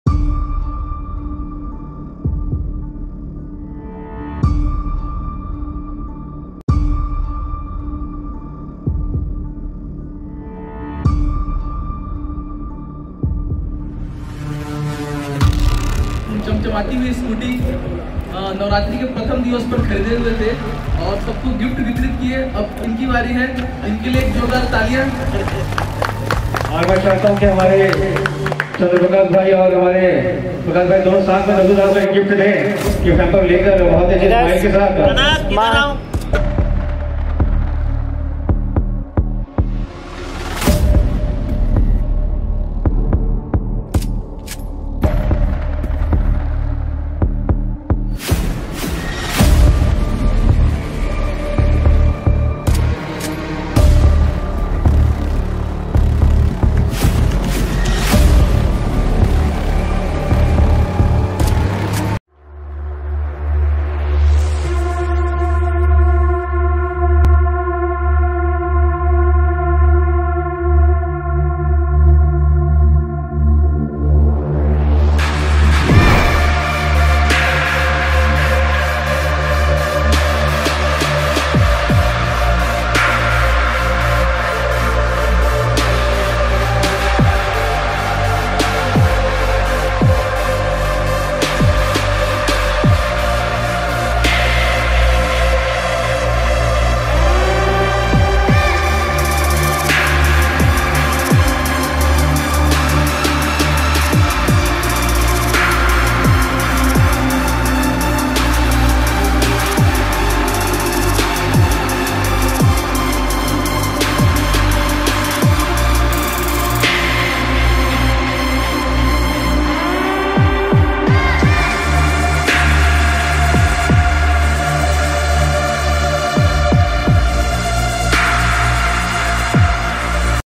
Humare Customers ke liye Lucky Draw !! 🥳🥳. Electric scooty ki kharidi par Exciting prizes⚡️⚡️
Humne celebrate 🥳🥳 kiya humara festival 🪔humare Happy customers ke sath !!!
Is festive season mein jisne bhi humare shop se electric scooty ⚡️liya, use mila nishchit uphar 🥳🥳!!
Dekhiye is video mein humne or humare customers ke kuch haseen lamhe 😊😊

Agar aap bhi chahte hai Affordable Electric scooty to aaj hi visit kare test ride ke liye

📍 Beside PG College, MG Road, Ambikapur (CG)